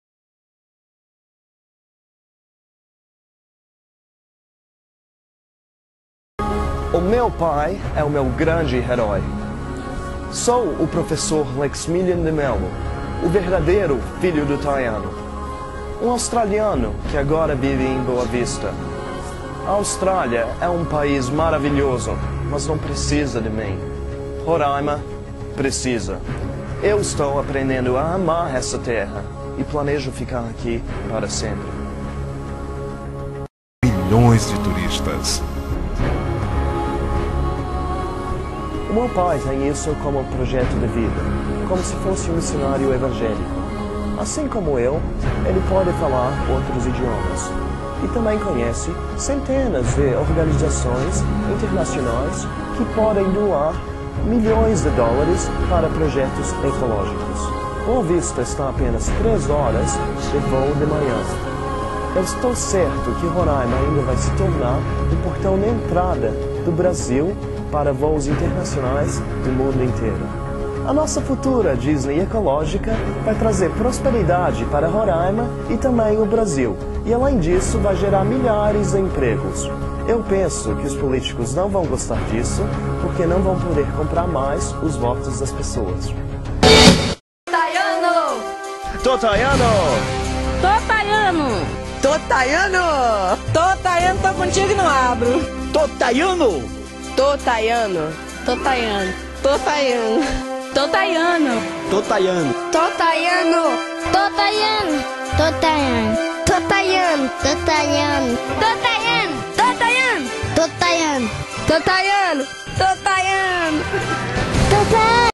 Lex's Part TAIANO 2004
Federal politics in 2004 were fierce, this in the Amazonian region in Roraima (R.R.).  I had to drop out of studies, and work, in NSW in order to teach English in the Amazon and help dad in political runnings for Governor of state.  This is a short clip, of a one hour documentary about my dad (and political advertising), of which portrays my televised speeches to the state.  Unfortunately our opponents resorted to foul play, kidnappings, extortion, bribery, and much corruption scandals - we lost!